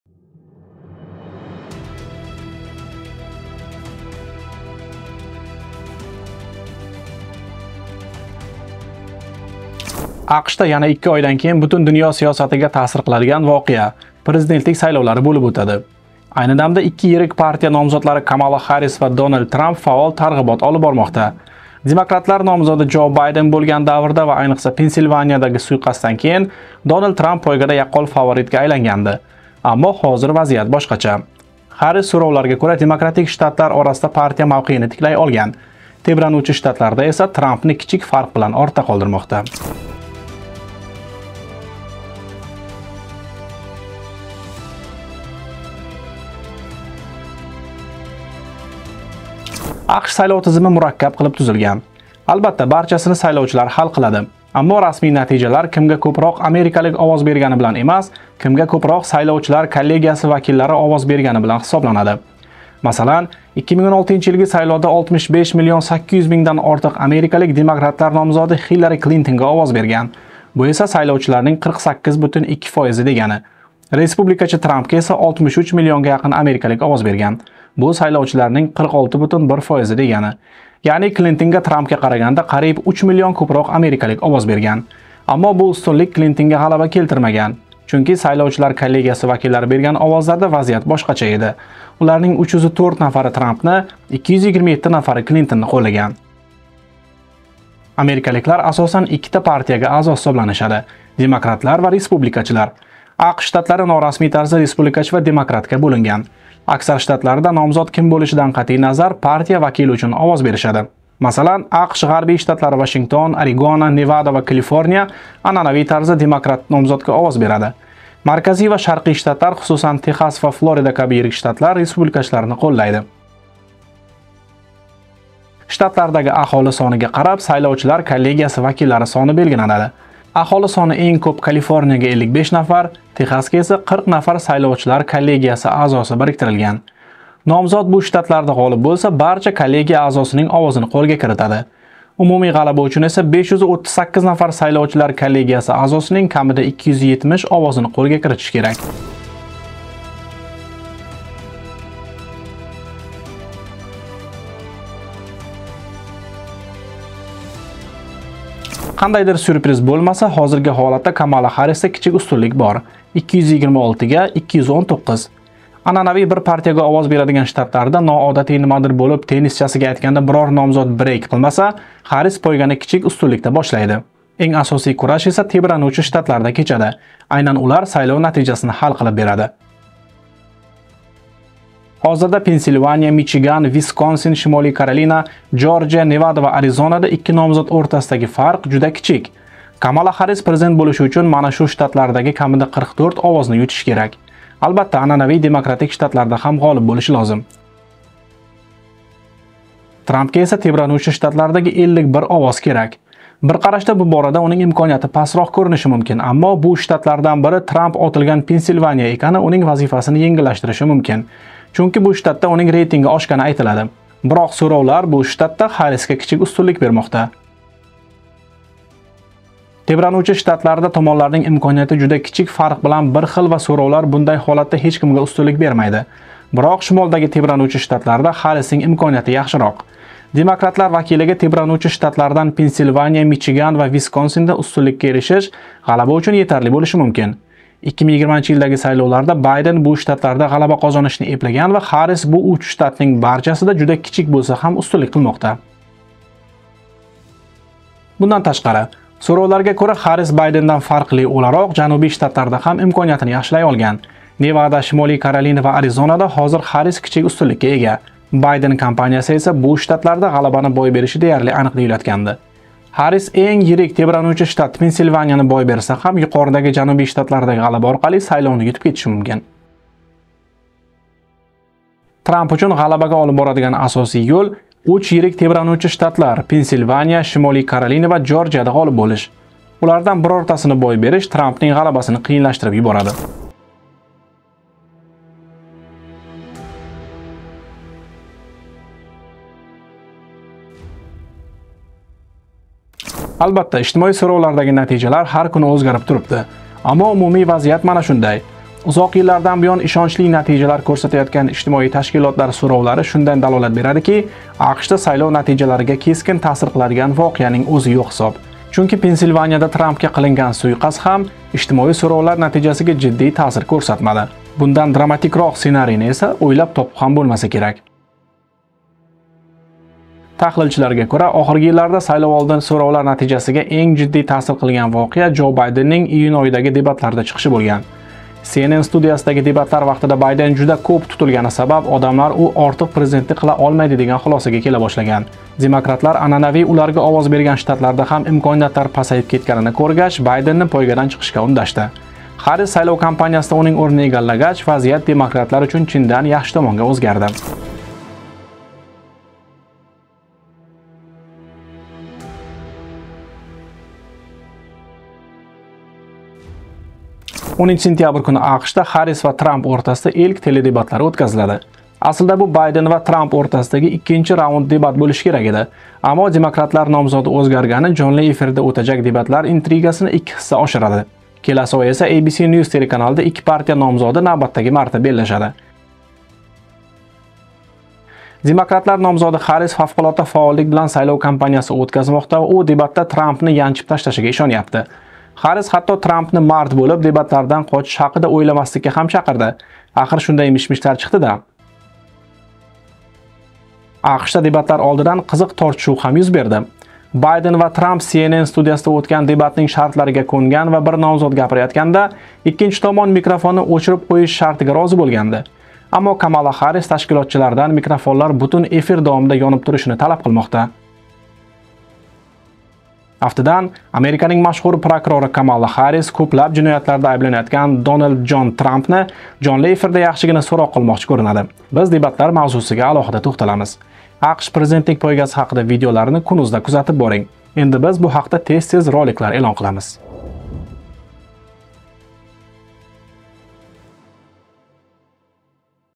AQSHda saylov yaqin: so‘rovlar Harrisga ustunlik bermoqda
Jo Bayden poygadan chiqqach, Kamala Harris demokratik shtatlarda partiya mavqeyini tiklay oldi. Ikkilanuvchi shtatlarda esa Trampdan ustunlik qilmqoda.